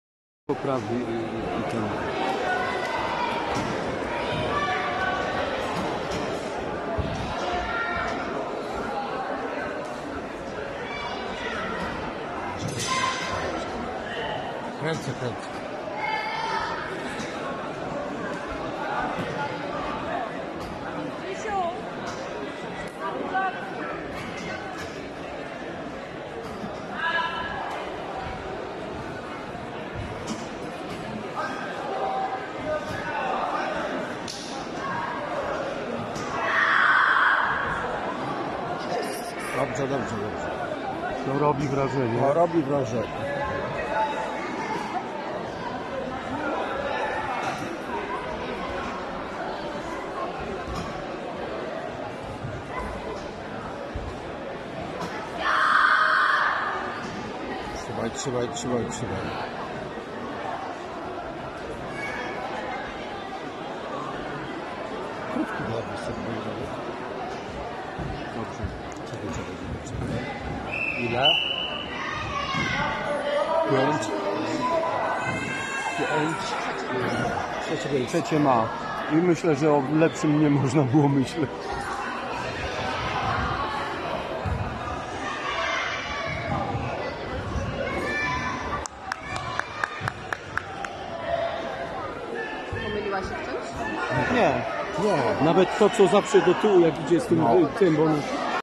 Mistrzostwa Polski w sztukach walki Federacji IMAF Polska 2021 w Bukownie start naszej zawodniczki .
W konkurencji kata karate martial arts national championships na w/w mistrzostwach nasza zawodniczka wywalczyla brazowy medal w kategorii 13-latkow.